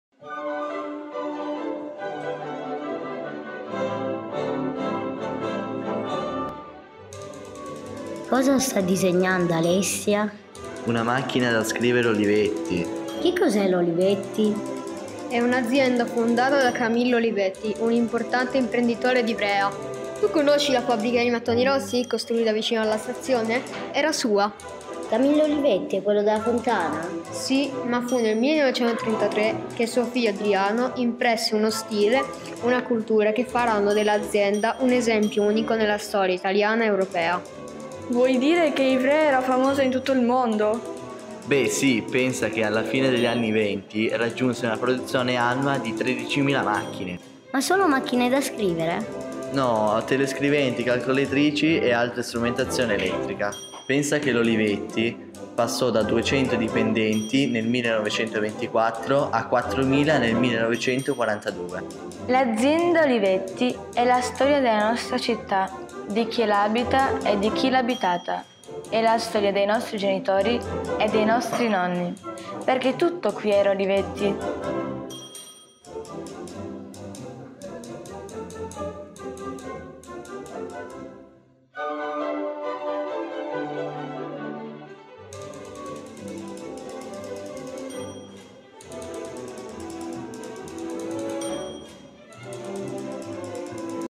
Animazione Olivetti, innovazione e futuro
Olivetti, futuro e innovazione
è una video animazione realizzata durante il laboratorio estivo “La fabbrica delle storie”.
Organizzato dal museo Tecnologicamente di Ivrea, interamente sceneggiata, disegnata e interpretata dai ragazzi che hanno svolto il laboratorio sotto la guida di Corrado Bianchetti,  presso la Scuola Media Statale G. Falcone